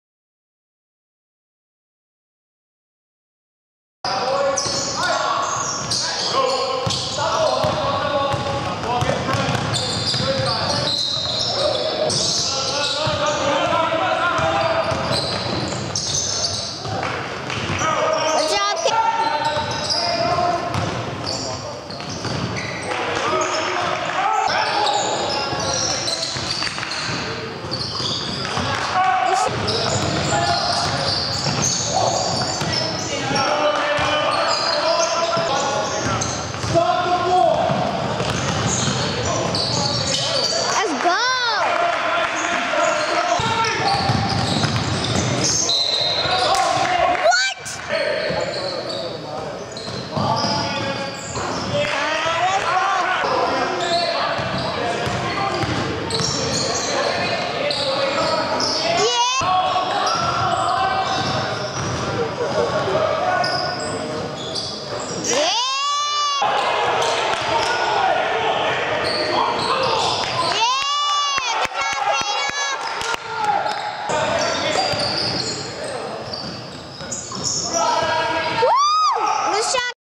EL Khana High School High Lights
Class of 2019 El Khana Hidalgo playing in a high school Game. El Khana average 22 points playing high school basketball while still in the 8th grade.